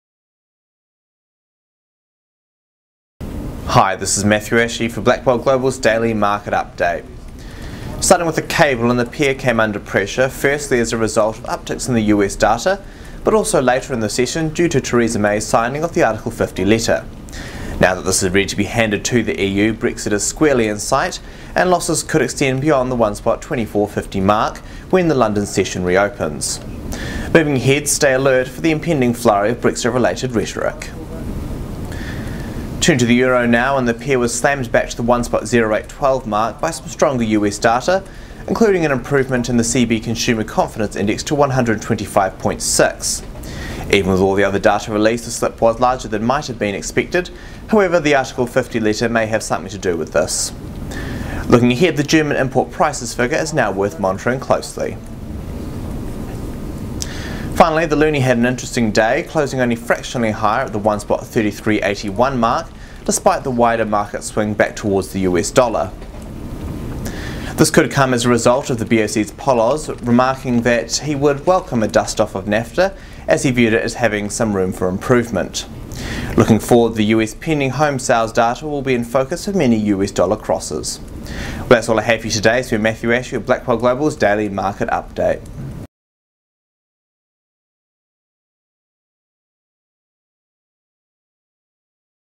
29th March 2017 – Brexit Inches Ever Closer as May Signs the Article 50 Letter
In today’s market update, we take a look at what happened to the cable given the latest development in the Brexit saga. Additionally, we talk about the EUR and what factors caused it to have such a notable tumble in the prior session. Finally, we discuss the Loonie and how the BoC’s Poloz influenced the pair with his latest remarks.